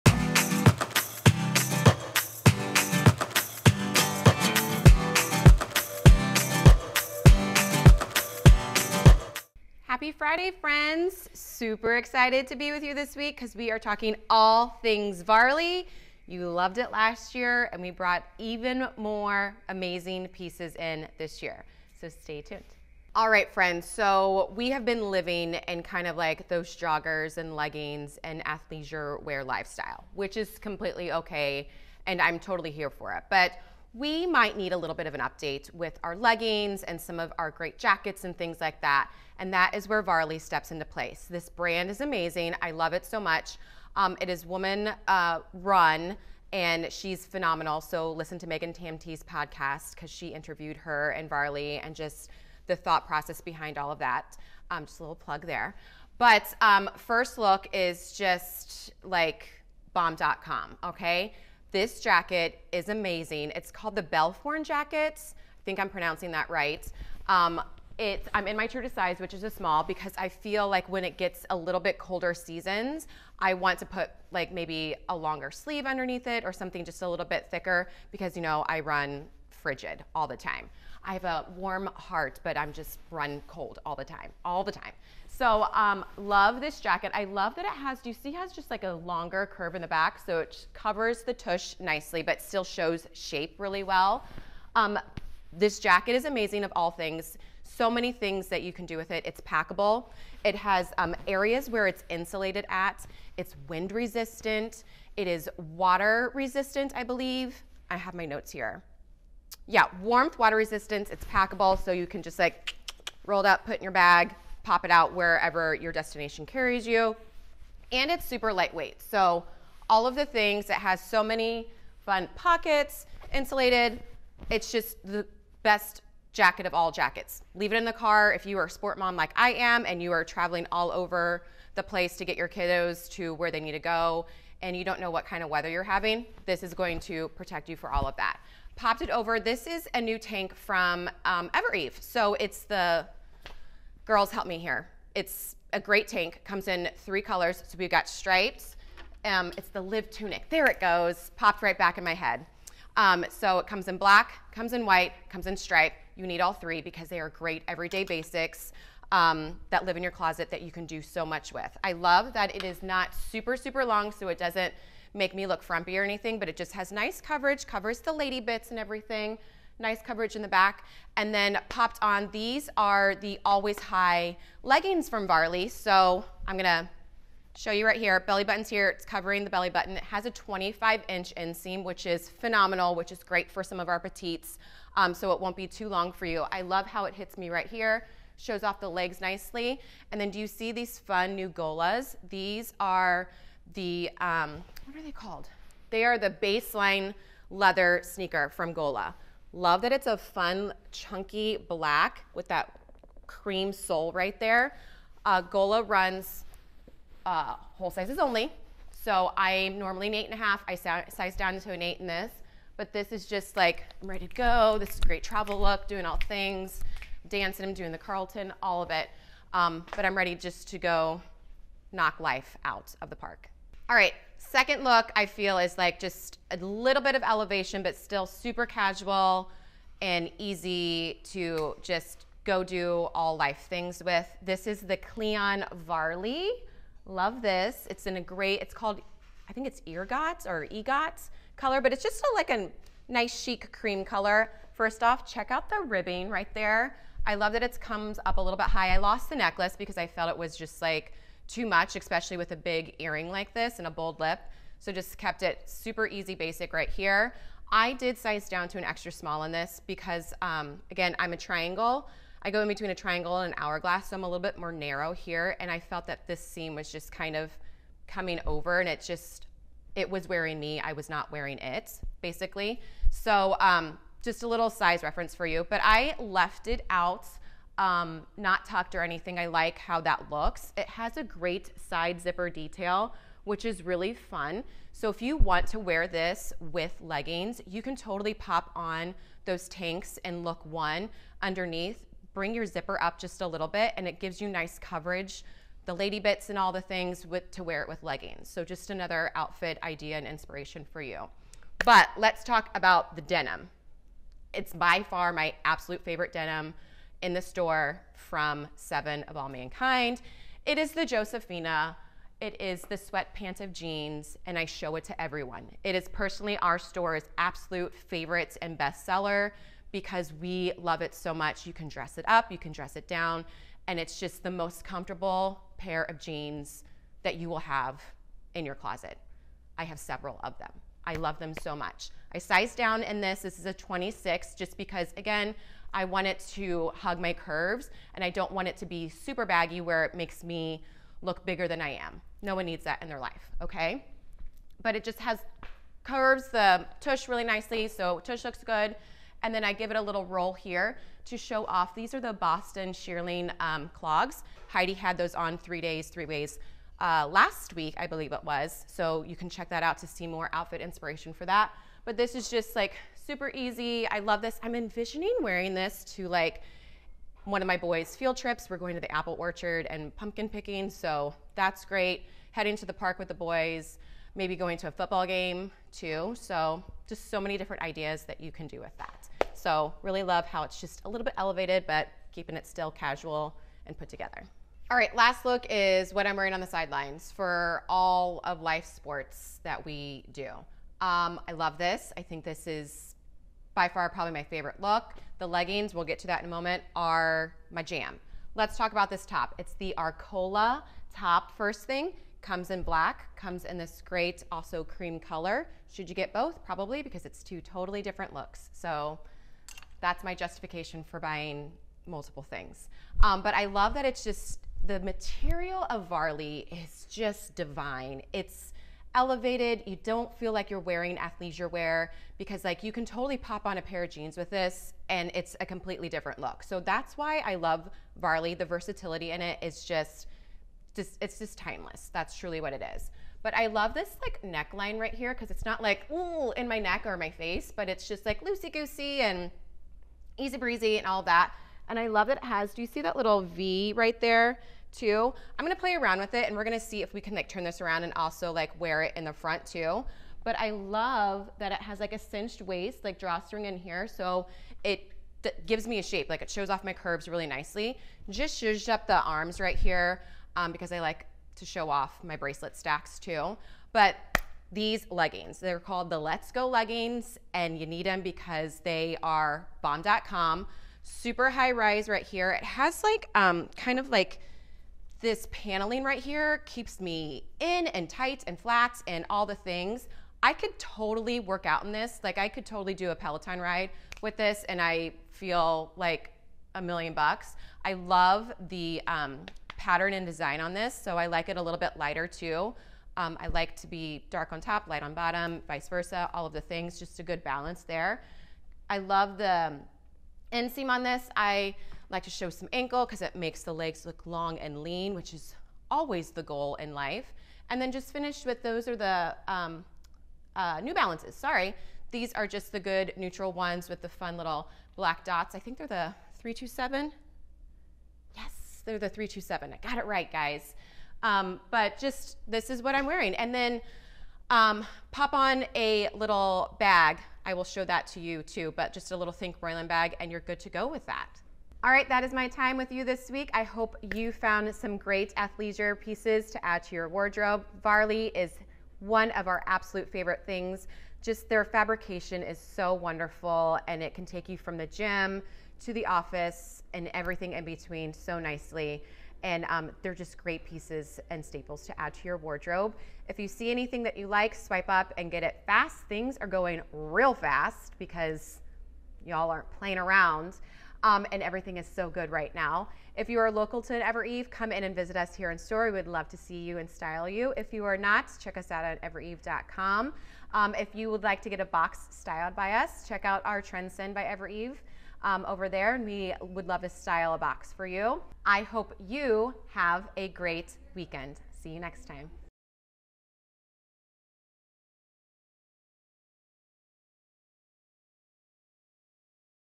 All Things Varley
The fashleisure brand you love is BACK with new styles for the season. Tune in as Meg shows us how to style comfy-chic Varley pieces with favorites from your fall wardrobe. Item links & details below!

See the full selection of clothing and shoes featured in this video at EVEREVE.com

 0:00 // Intro

 0:10 // Look 1

3:42 // Look 2

6:24 // Look 3